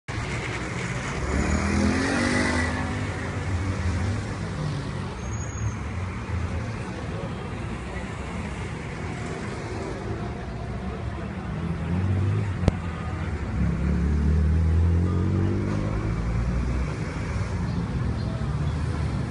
Arrollan a ciclista en Av. Universidad.
Una ciclista resultó lesionada al ser arrollada por vehículo que buscaba incorporarse a Universidad. El hecho ocurrió entre Gutiérrez Nájera y Altamirano. Paramédicos de Cruz Roja arribaron al lugar para brindarle atención.